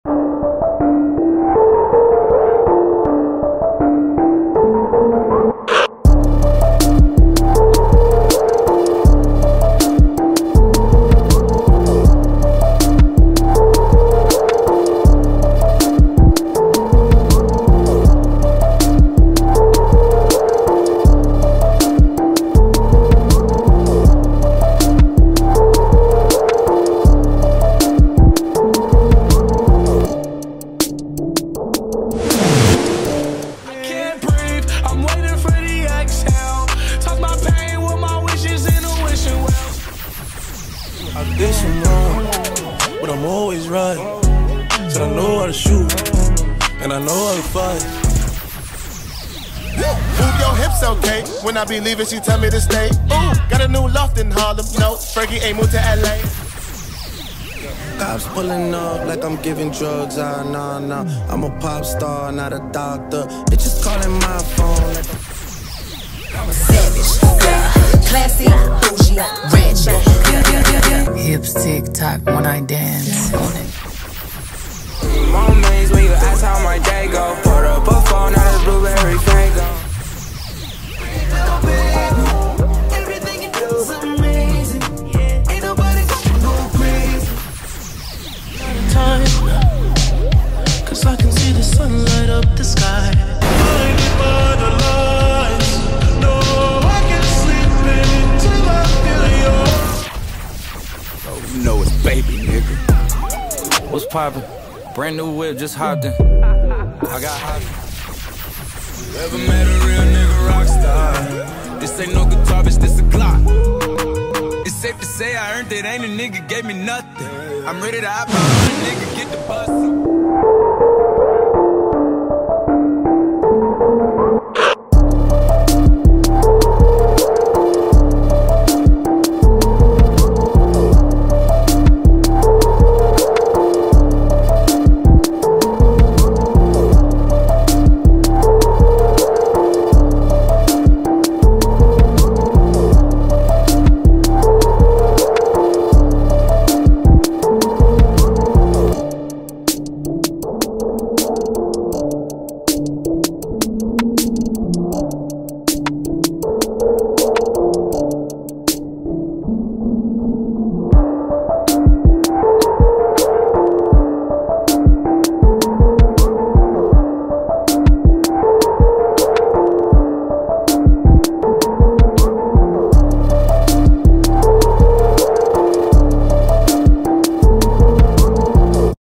Billboard Hot R&B/Hip-Hop Songs - August 15th, 2020 | Top 10 R&B/Hip-Hop Songs of the Week
These are the Top 10 songs on Billboard's Hot R&B/Hip-Hop songs chart for the week in the United States, as ranked and compiled by Nielsen Music, for the chart dated August 15th, 2020 (Week 32).

Check out all of our latest videos.